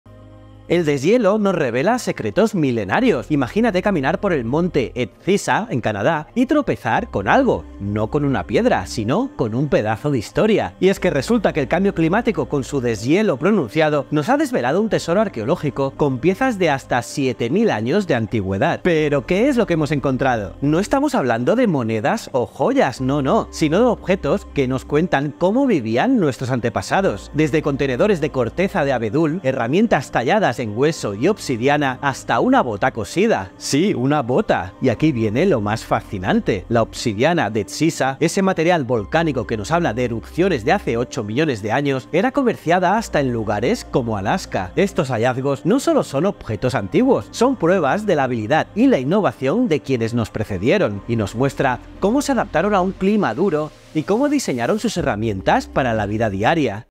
¡Increíble Hallazgo bajo el Hielo Canadiense Revela Tesoros de 7 Milenios!
Exploramos el sorprendente hallazgo en el monte Edziza, Canadá, donde el cambio climático ha desvelado un tesoro arqueológico de hasta 7.000 años de antigüedad. Descubrimos objetos como contenedores de corteza de abedul, herramientas de hueso y obsidiana, e incluso una bota antigua. Este vídeo destaca la importancia de la obsidiana de Edziza, comerciada hasta en Alaska, y reflexiona sobre la habilidad e innovación de nuestros antepasados, su adaptación a climas duros y el diseño de herramientas para la vida cotidiana.